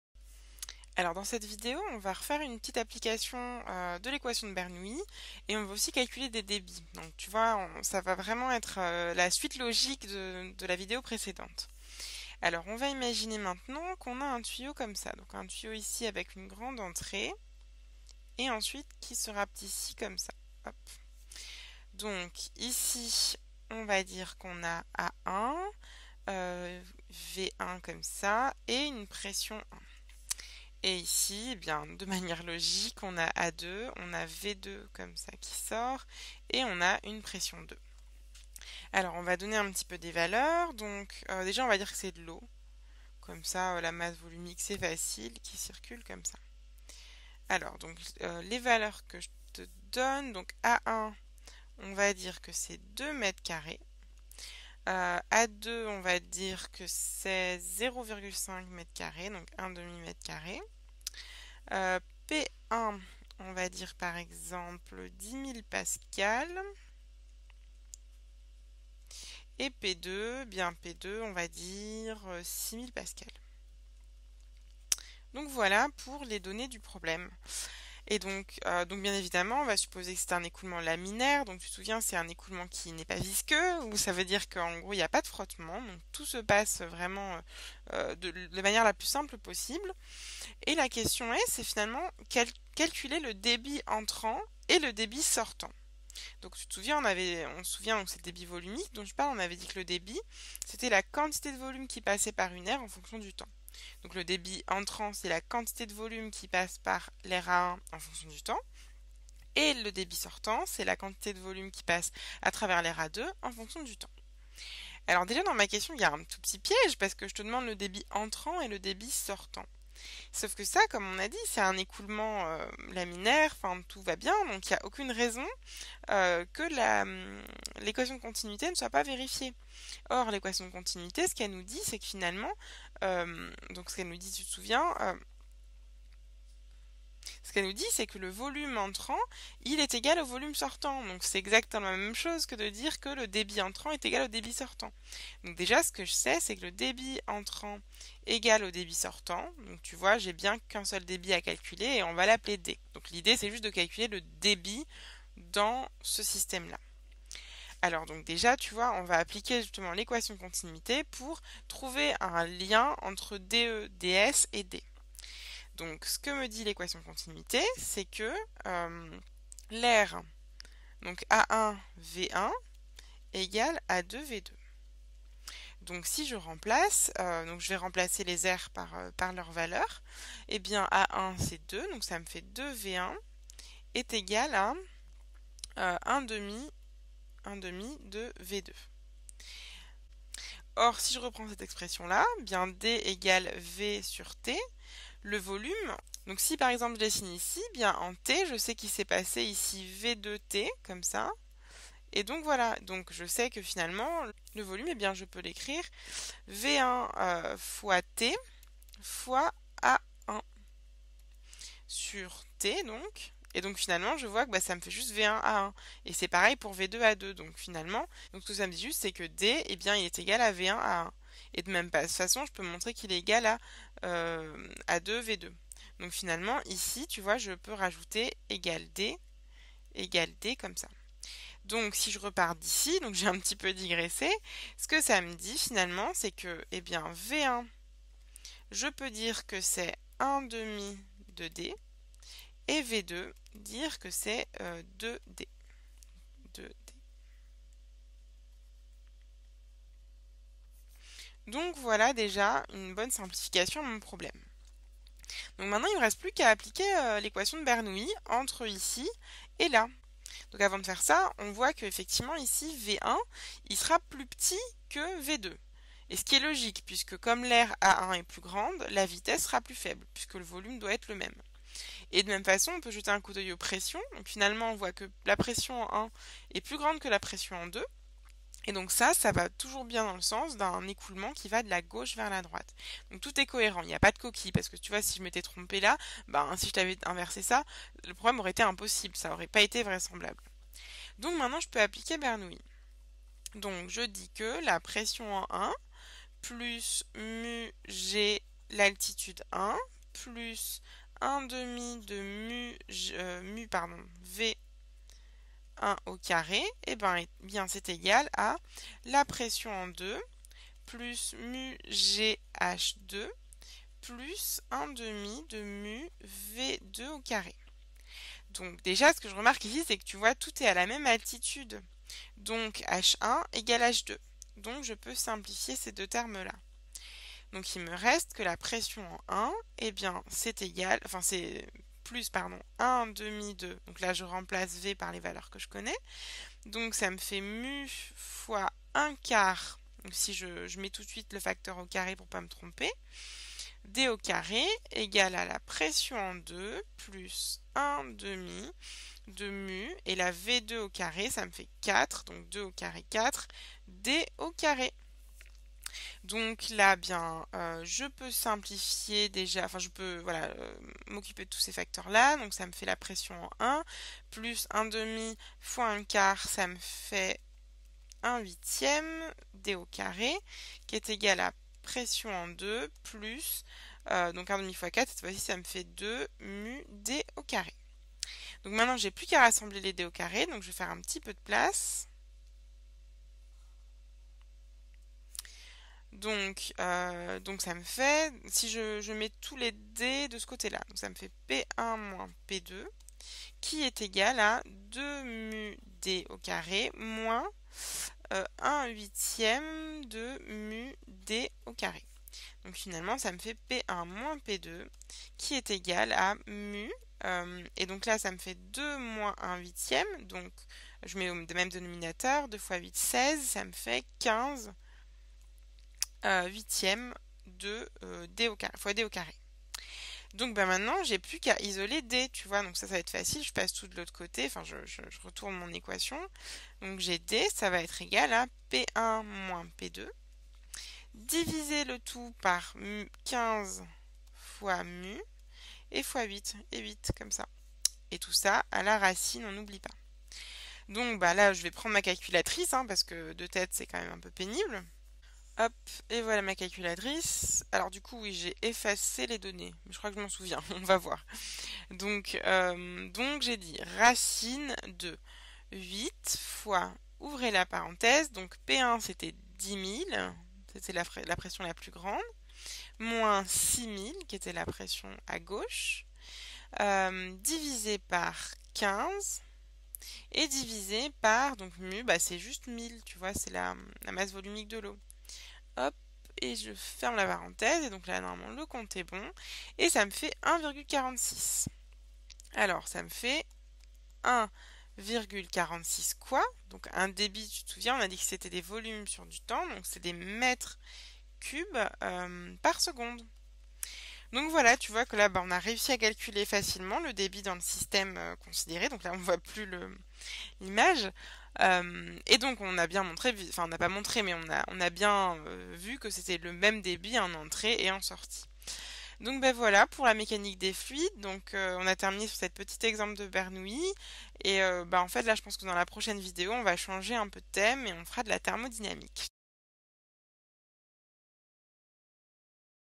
Application de l'équation de Bernoulli : Calcul d'un débit volumique
Calcul de débits volumiques dans un tuyau de section variable